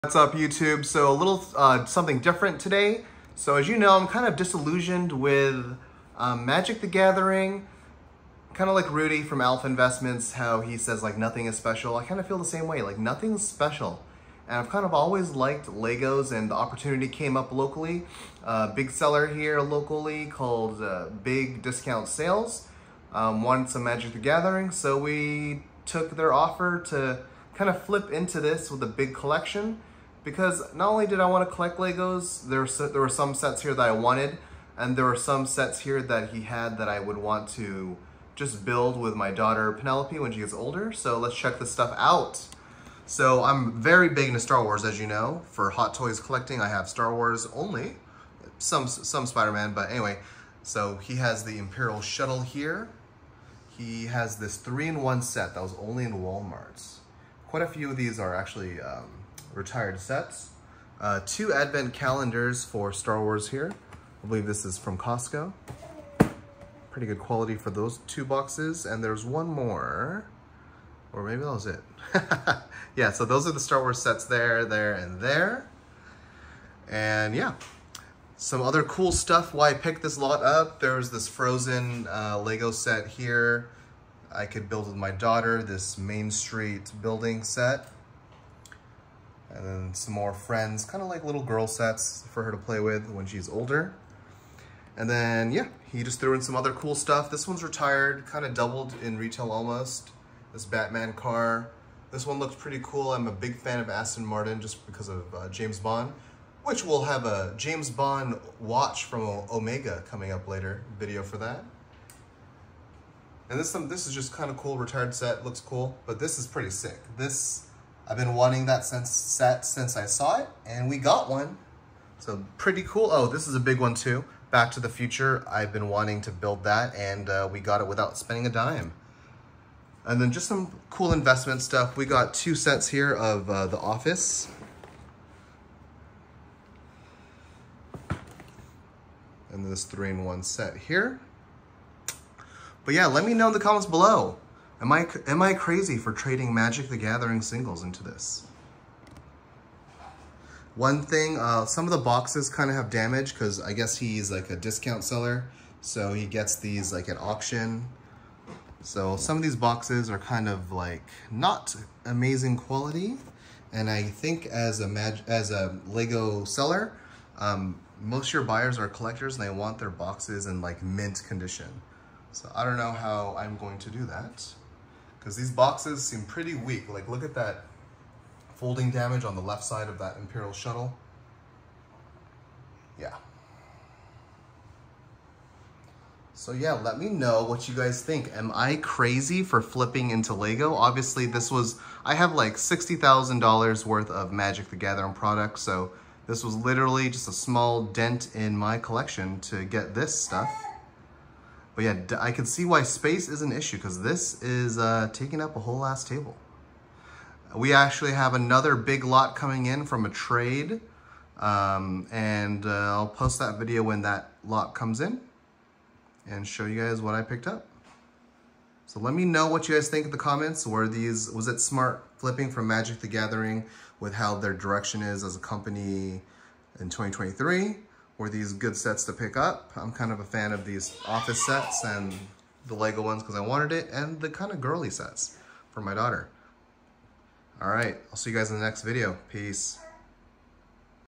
Trading Magic the Gathering for LEGO sets
Been losing interest in Magic the Gathering for awhile. Been playing with the idea of collecting LEGO sets. A local ebay seller reached out to offer a trade for his LEGO for some MTG and it escalated quickly. Check him out at his ebay below: